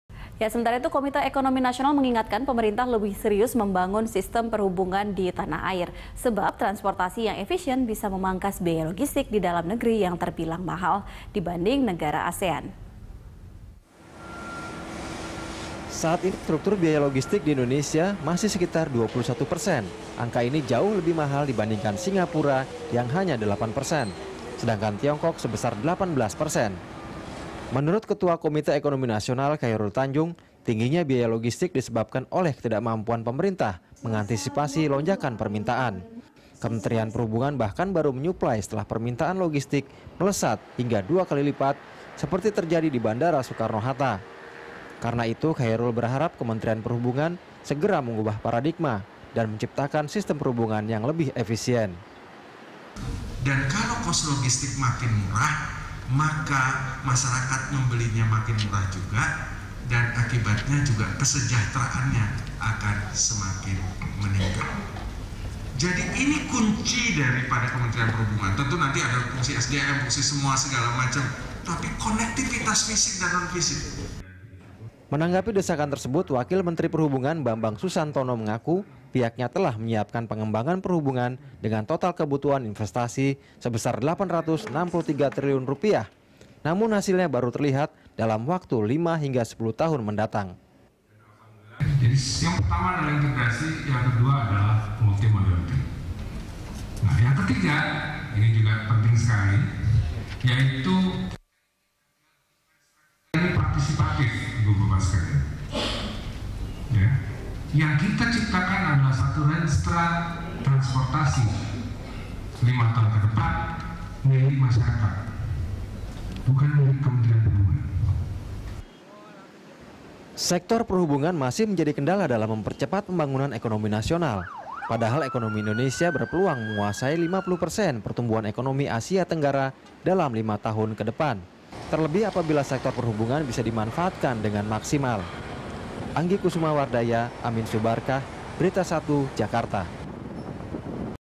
Pemerintah Didesak Mempercepat Pembangunan Sistem Perhubungan Efisien
Komite Ekonomi Nasional (KEN) mendesak Pemerintah serius membangun sistem perhubungan di Tanah Air. Sebab, transportasi yang efisien bisa memangkas biaya logistik di dalam negeri yang terbilang mahal dibanding negara ASEAN.

Facebook.com/BeritaSatuTV
Youtube.com/BeritaSatu